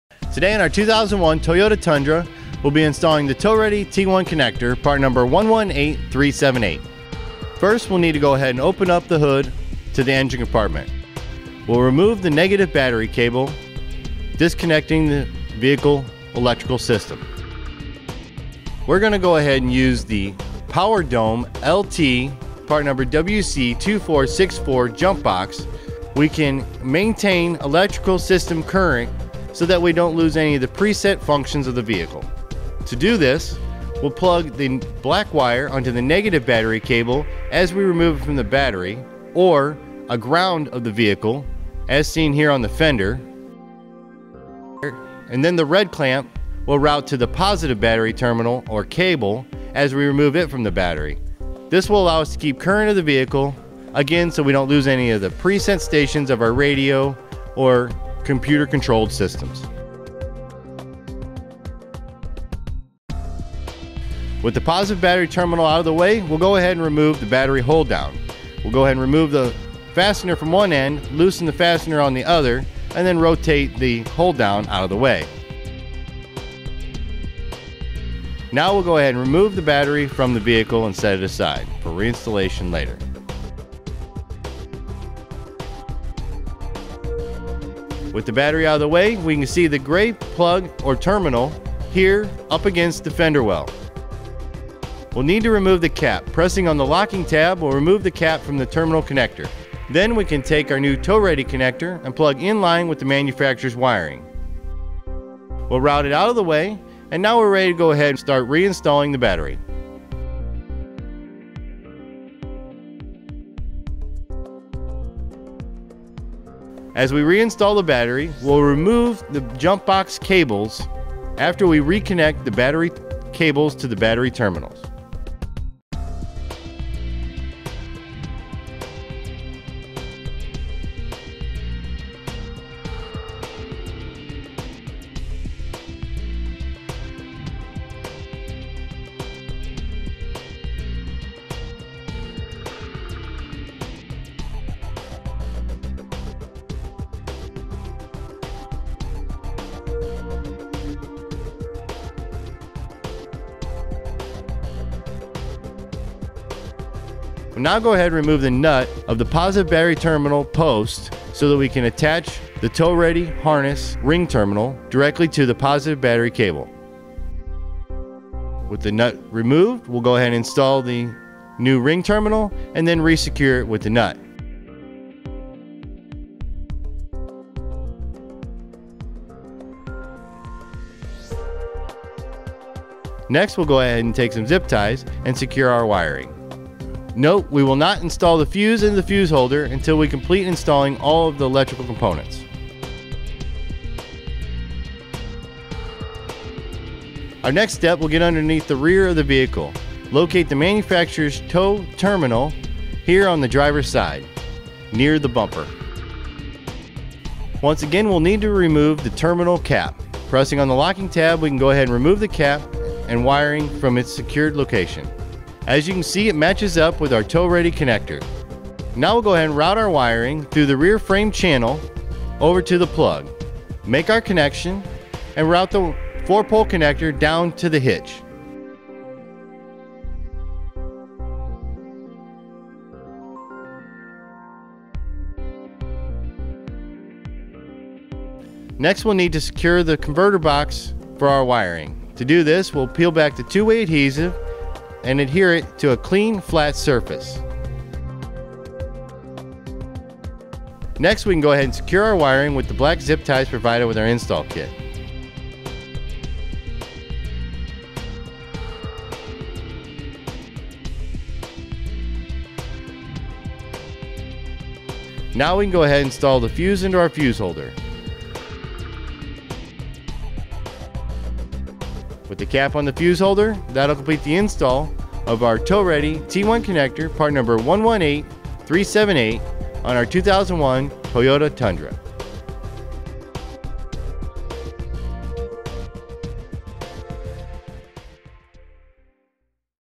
Mount: T-One Vehicle Wiring Harness with 4-Pole Flat Trailer Connector on a 2001 Toyota Tundra
Today on our 2001 Toyota Tundra well be installing the Tow Ready T1 connector, part number 118378.  First well need to go ahead and open up the hood to the engine compartment.  Well remove the negative battery cable, disconnecting the vehicles electrical system.  Were going to go ahead and use the Power Dome LT, part number WC2464 jump box.  We can maintain electrical system current so that we dont lose any of the preset functions of the vehicle.  To do this well plug the black wire onto the negative battery cable as we remove from the battery, or a ground of the vehicle as seen here on the fender, and then the red clamp well route to the positive battery terminal, or cable as we remove it from the battery.

This will allow us to keep current in the vehicle, again so we dont lose any of the preset stations of our radio or computer control systems. With the positive battery terminal out of the way, well go ahead and remove the battery hold down.  Well go ahead and remove the fastener from one end, loosen the fastener on the other and then rotate the hold down out of the way.  Now well go ahead and remove the battery from the vehicle and set it aside for reinstallation later.  With the battery out of the way we can see the grey plug, or terminal here up against the fender well.

Well need to remove the cap.  Pressing on the locking tab, well remove the cap from the terminal connector.  Then we can take our new Tow Ready connector and plug in line with the manufacturers wiring.  Well route it out of the way and now were ready to go ahead and start reinstalling the battery.  As we reinstall the battery well remove the jump box cables after we reconnect the battery cables to the battery terminals.

Well now go ahead and remove the nut of the positive battery terminal post so that we can attach the Tow Ready harness ring terminal directly to the positive battery cable.  With the nut removed well go ahead and install the new ring terminal and then resecure it with the nut.  Next well go ahead and take some zip ties and secure our wiring.  Note, we will not install the fuse in the fuse holder until we complete installing all of the electrical components. Our next step well get underneath the rear of the vehicle, locate the manufacturers tow terminal here on the drivers side, near the bumper.

Once again, well need to remove the terminal cap.  Pressing on the locking tab we can go ahead and remove the cap and wiring from its secured location.  As you can see it matches up with our Tow Ready connector.  Now well go ahead and route our wiring through the rear frame channel over to the plug, make our connection and route the four pole connector down to the hitch. Next well need to secure the converter box for our wiring.  To do this well peel back the two-way adhesive and adhere it to a clean flat surface. Next we can go ahead and secure our wiring with the black zip ties provided with our install kit.  No we can go ahead and install the fuse into our fuse holder. With the cap on the fuse holder that will complete the install of our Tow Ready T1 connector, part number 118378 on our 2001 Toyota Tundra.